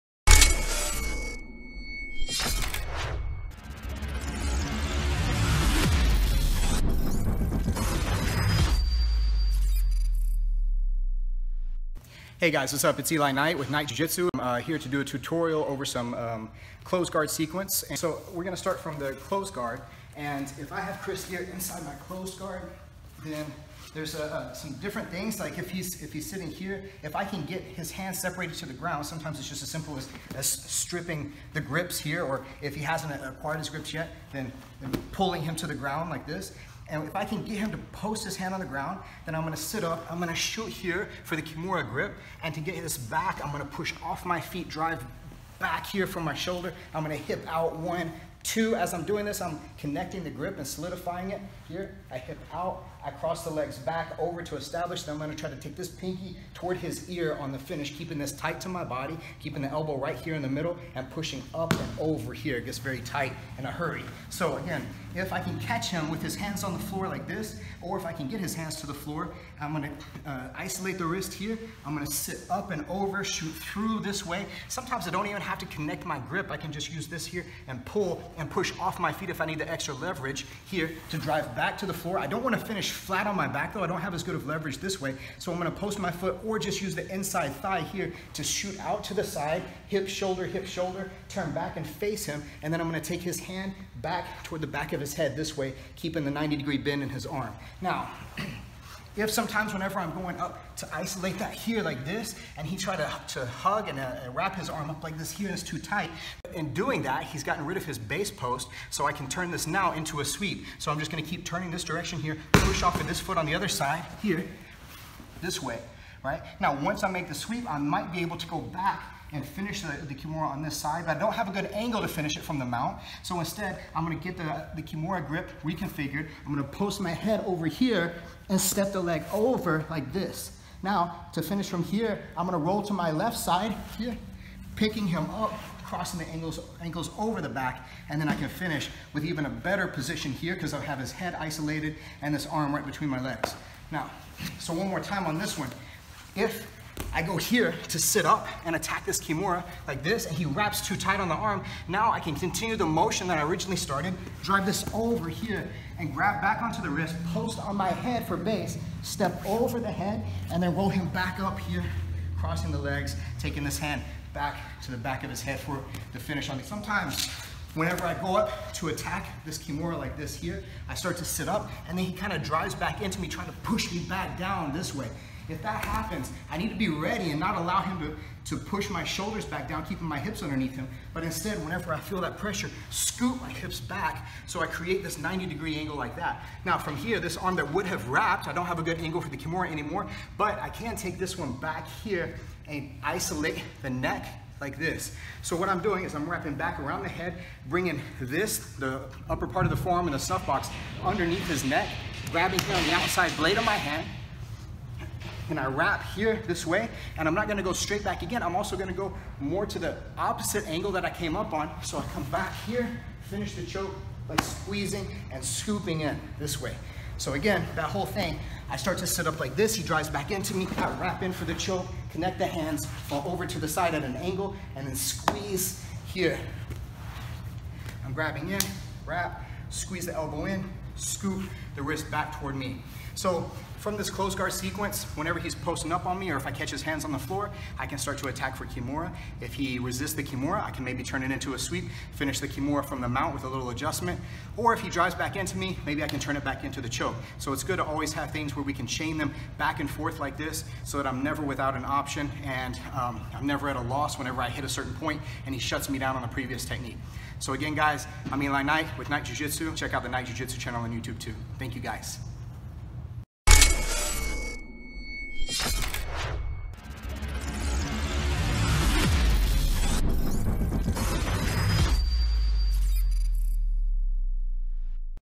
Jiu-Jitsu Submissions | Kimura, Sweep & Choke Sequence
Closed guard is making a comeback these days, and for good reason. It offers a lot of control along with attack options in combination like the ones in this video. This sequence is sure to elicit some responses that will lead to advancing your position, if not catching a submission.

Follow Aperture Fight Focused on FB and YT for the best training videos out there!